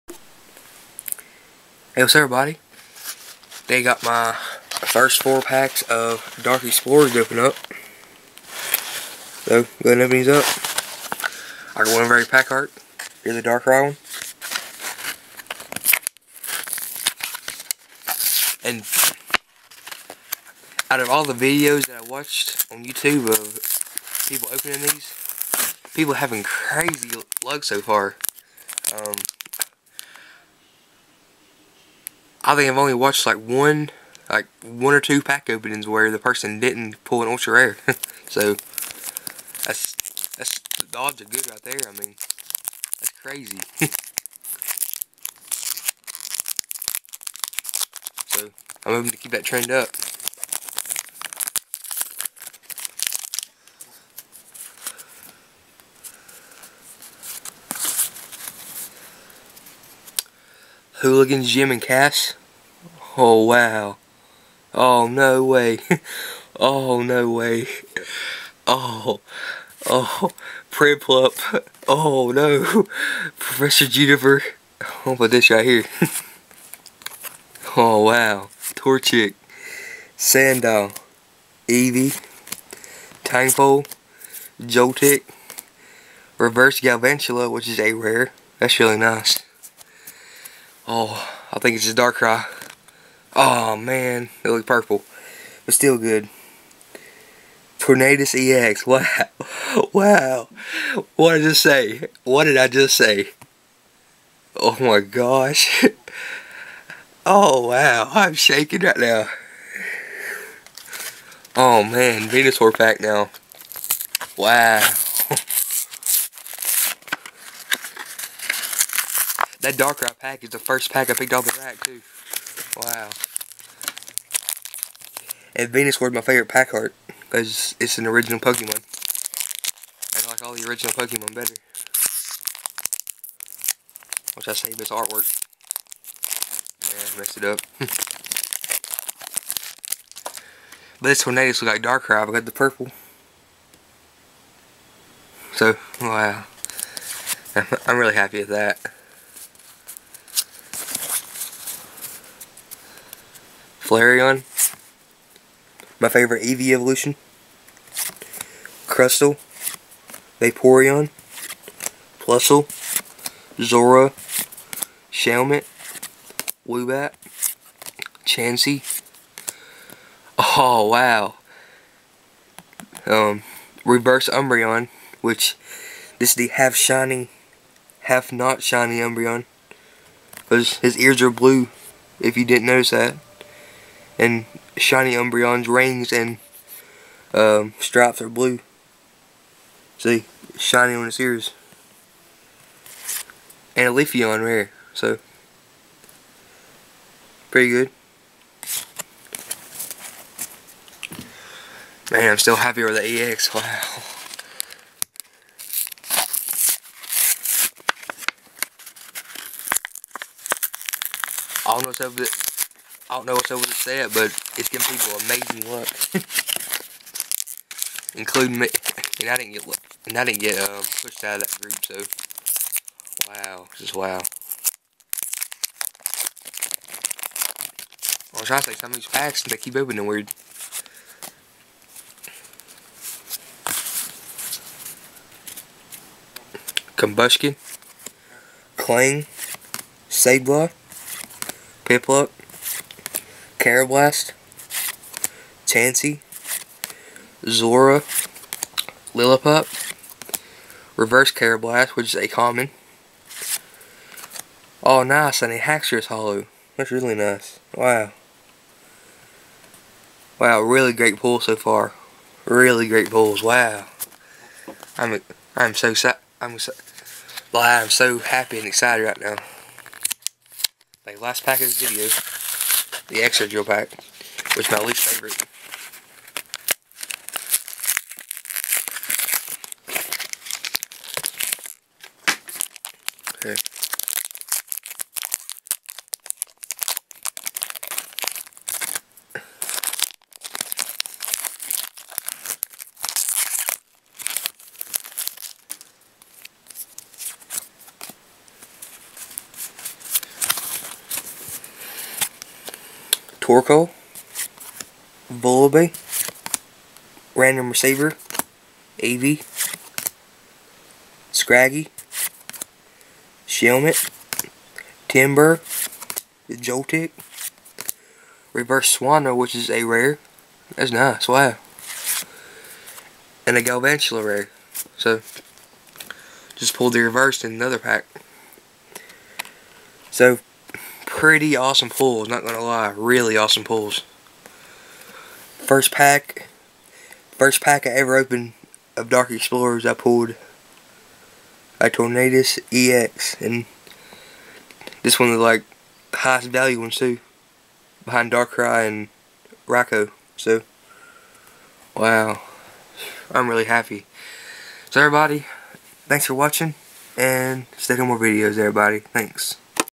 Opening 4 Packs Of Dark Explorers
Opening my first four packs of Dark Explorers
Pulled an EX !!!